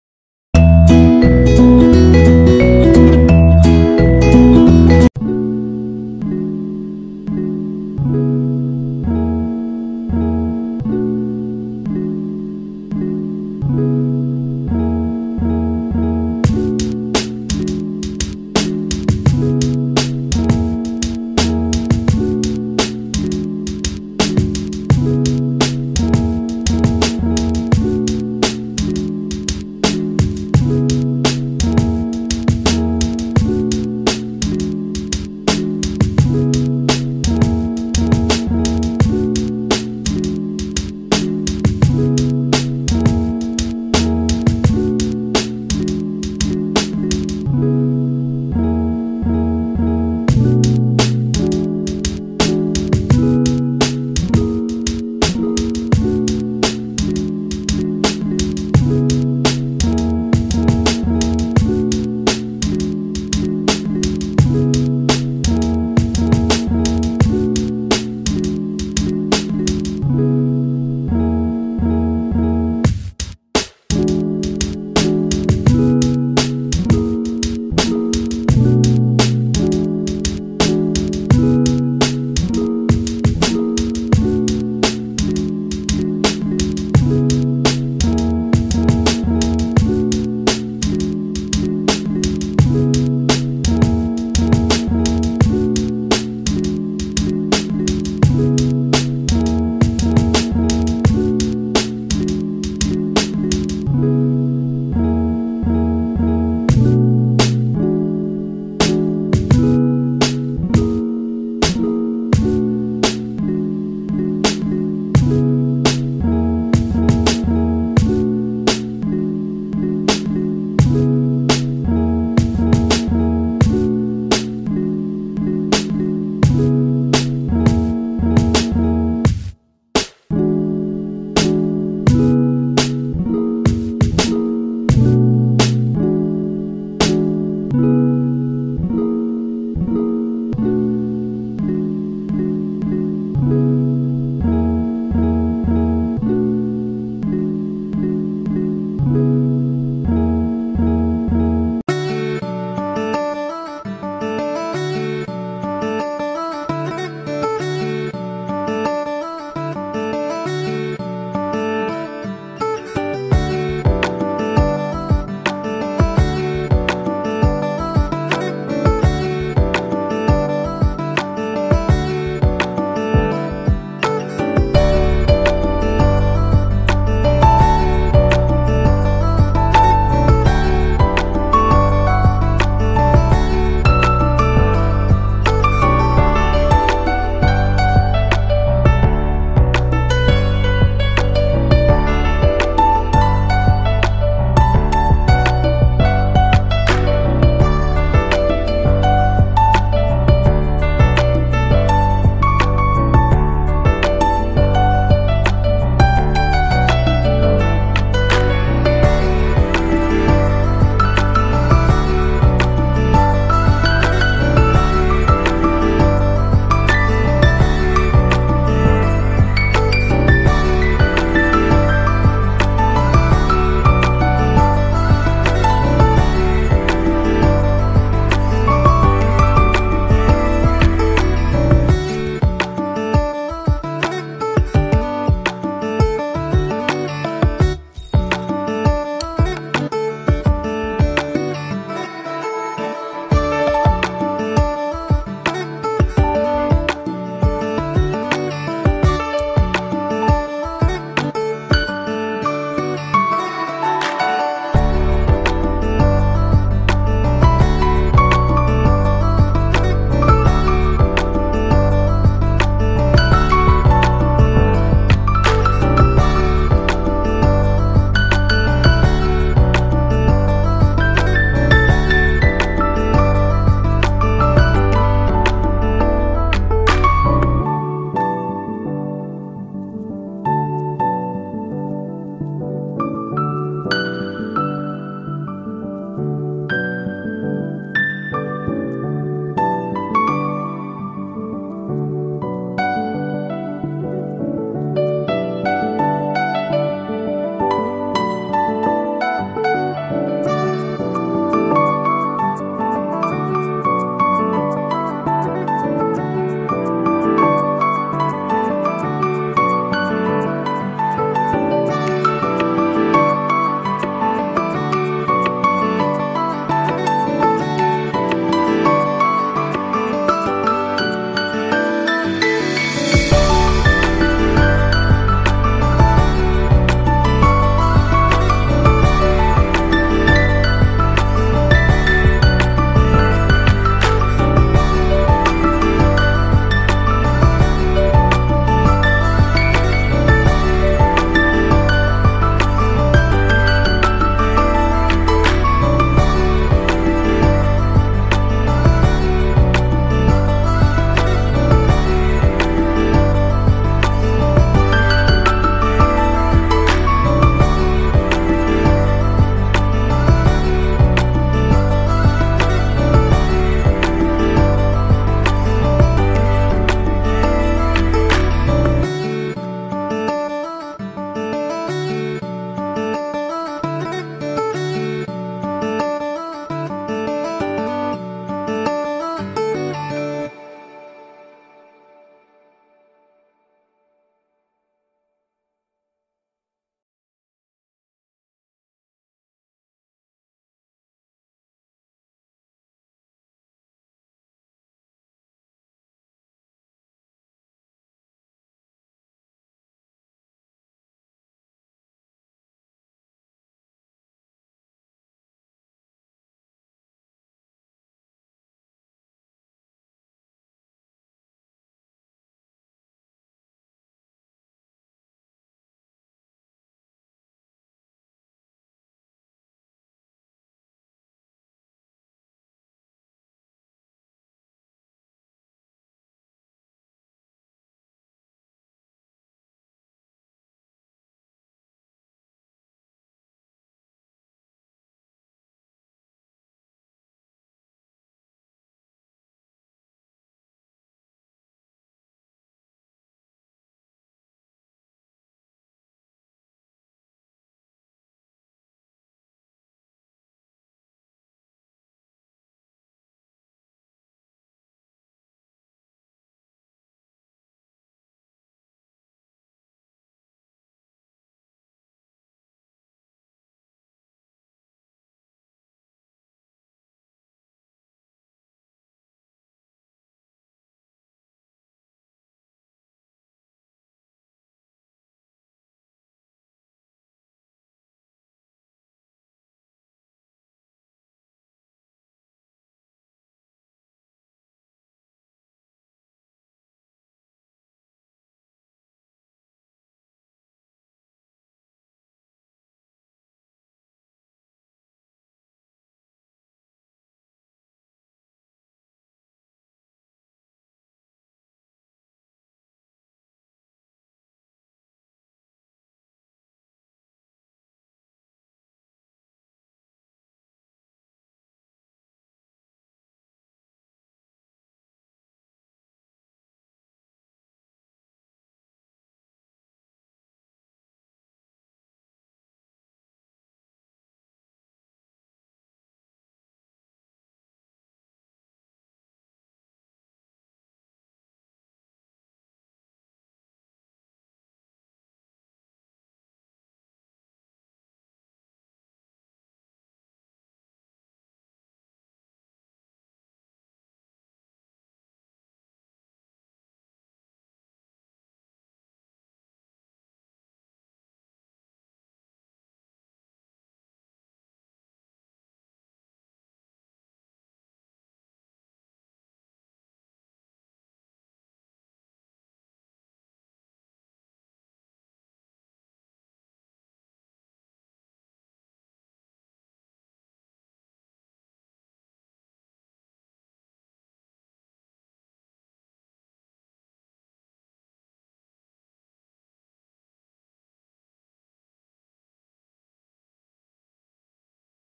How to draw 3D Square and Circle | Impossible Square and Cirle
How to draw a 3D Square and Circle for Kids easy and step by step